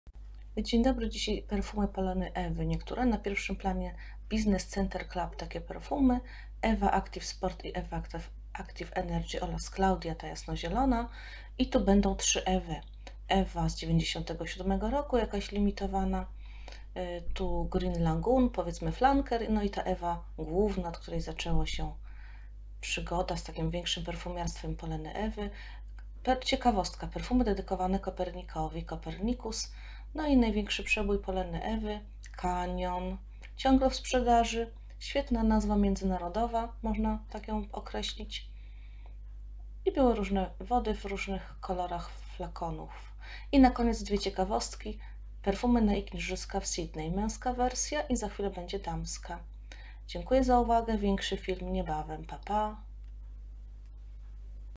Perfumy Polleny Ewy
Kilka flakonów perfum Polleny Ewy, które posiadam w domu.
Kolejno:
Businness Centre Club
Claudia
Eva Active Energy
Eva Active Sport
Eva Annual Collection 1997
Eva Green Lagoon
Eva
Copernicus
Kanion Classic
kanion Black
Kanion Active Sport
Kanion White
Sydney 2000 Eksplozja Wrażeń
Sydney 2000 Eksplozja Emocji